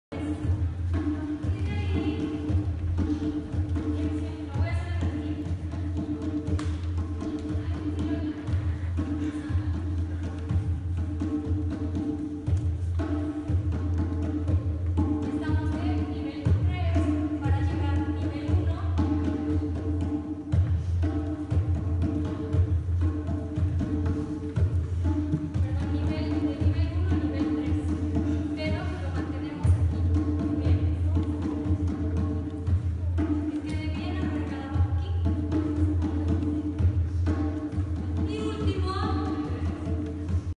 Clase de danza africana en la UNAM
Es una clase para actores , danza africana, bioenergética y proyección vocal.